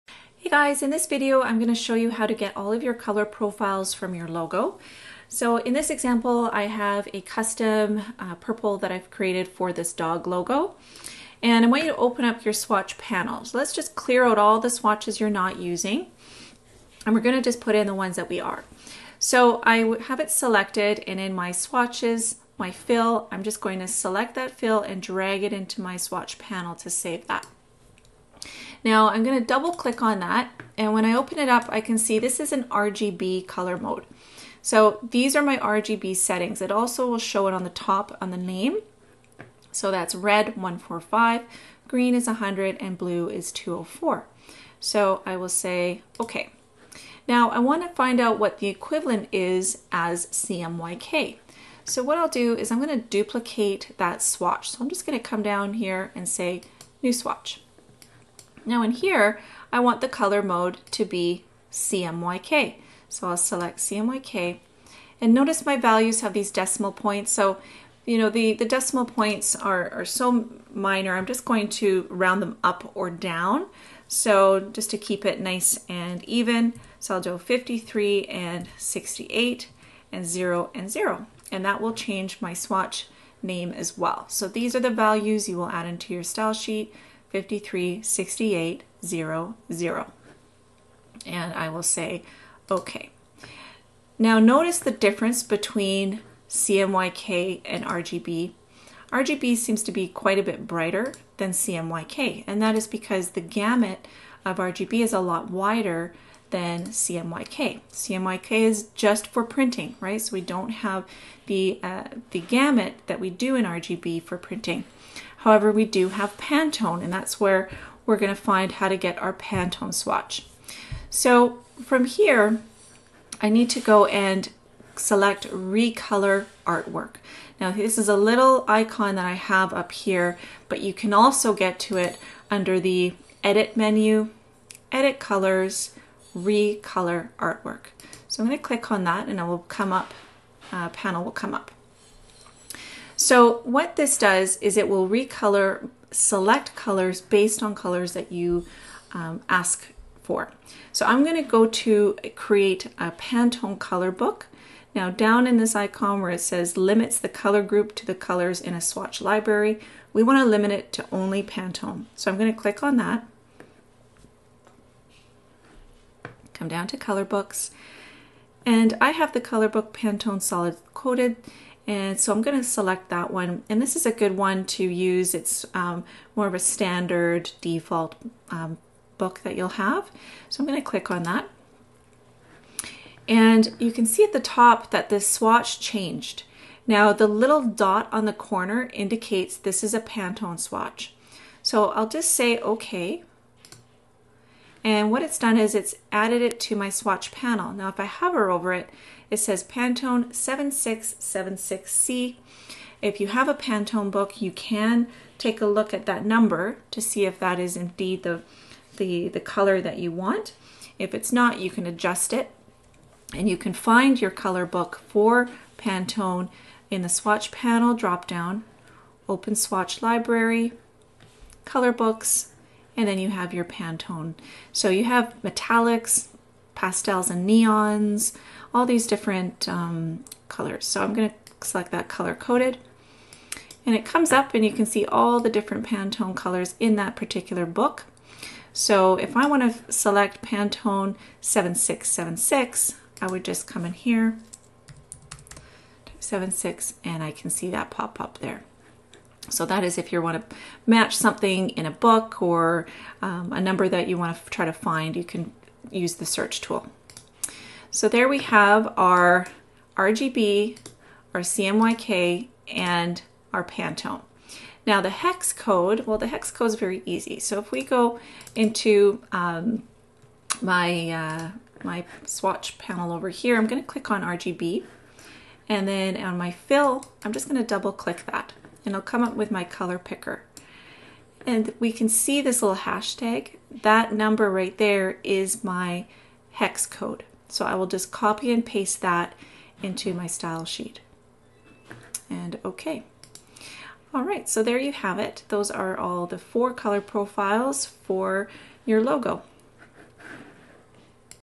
How to match a Pantone Color to a RGB or CMYK Color
In this video I will demonstrate how you can find all of the color profiles in your logo for your brand style sheet (RGB, CMYK, Pantone, Hex#) in Adobe Illustrator.